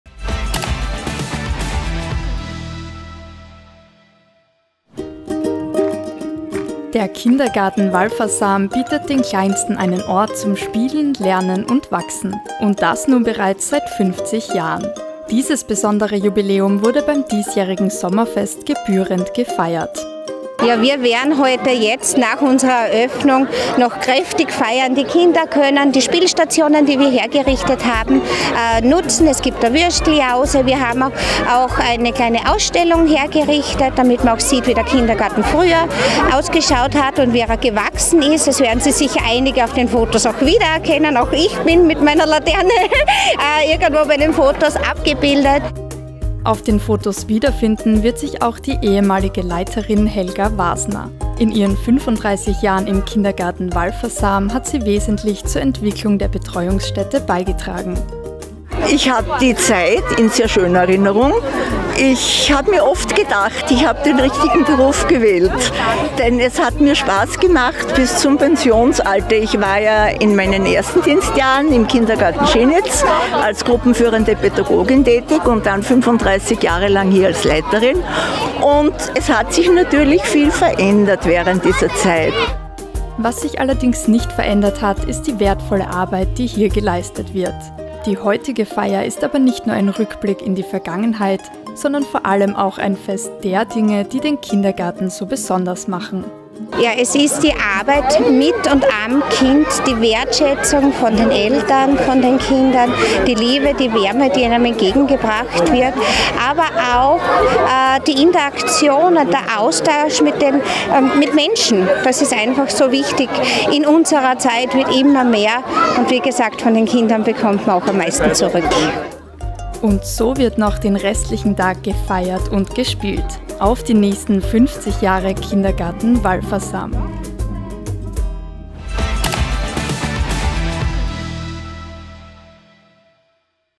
50 Jahre Kindergarten Walfersam
1974 wurde der Kindergarten in Kapfenberg-Walfersam eröffnet. Um dieses Jubiläum gebührend zu feiern gab es heute ein großes Fest. Spiel und Spaß kam dort für die Kleinsten nicht zu kurz.